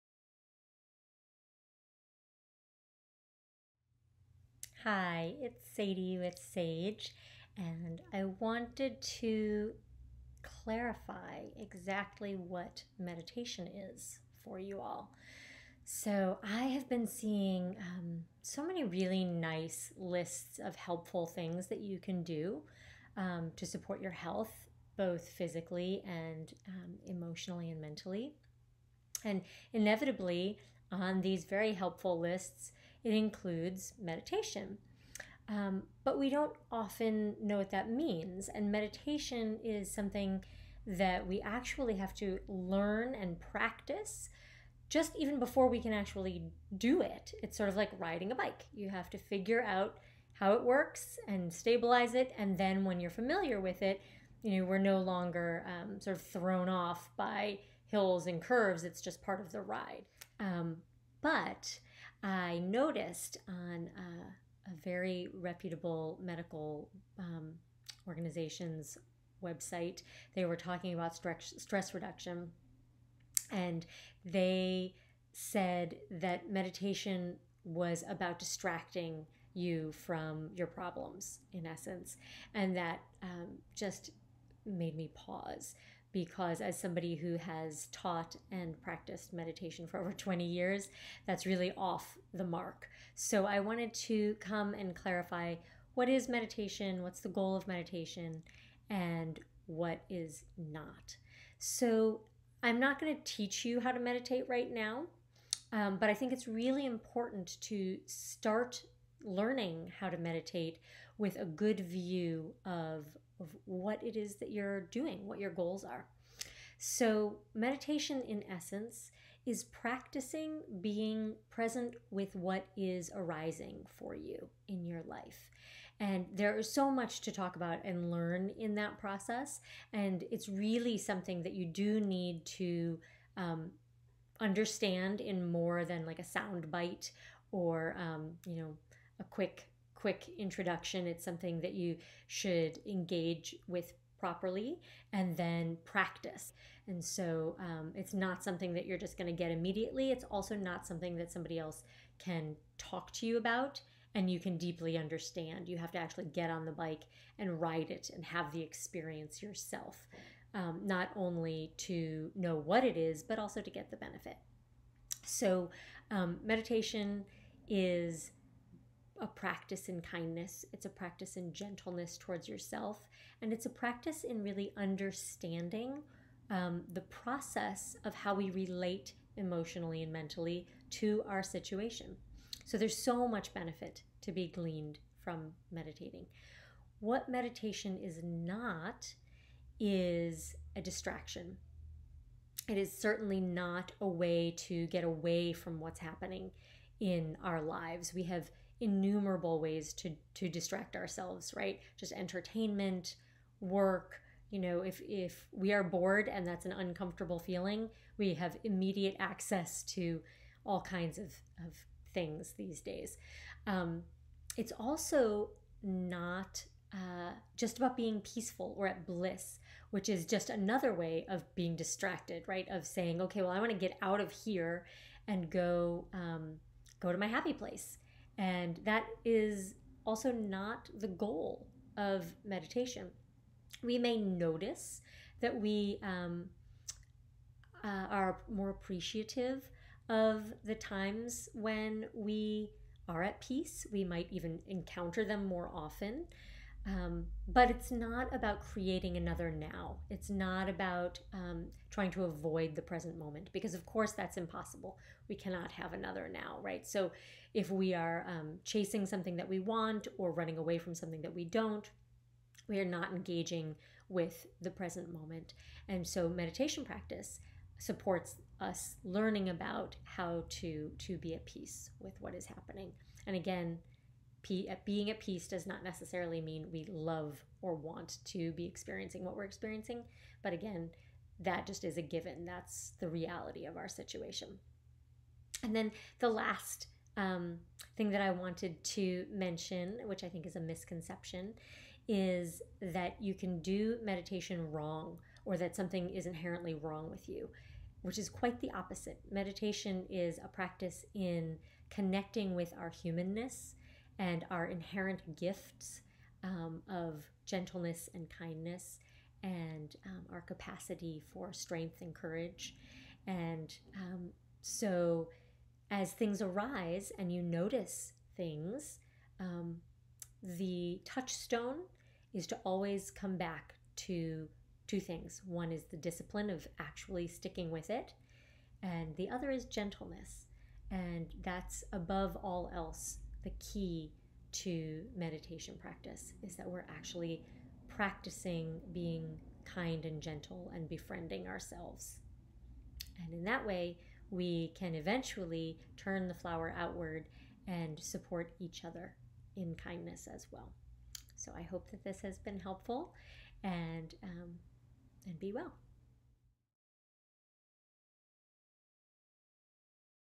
What is Meditation (and what it is not).
The biggest misconceptions about meditation.